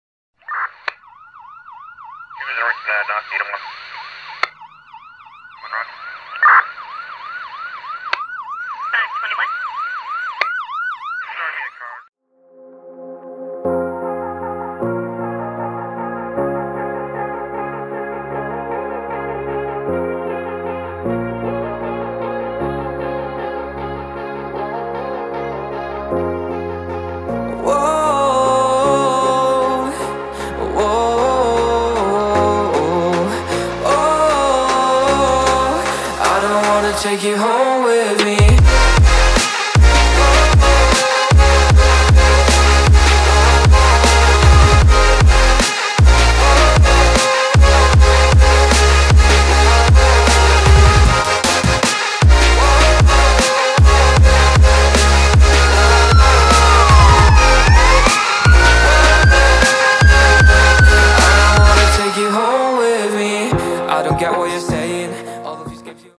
EMERLAN LANZAROTE ELIGE NUESTRO DELTA 35 ULTRAPLANO
Gracias a EMERLAN LANZAROTE que ha elegido nuestros DELTA 35 ultraplanos para equipar sus vehículos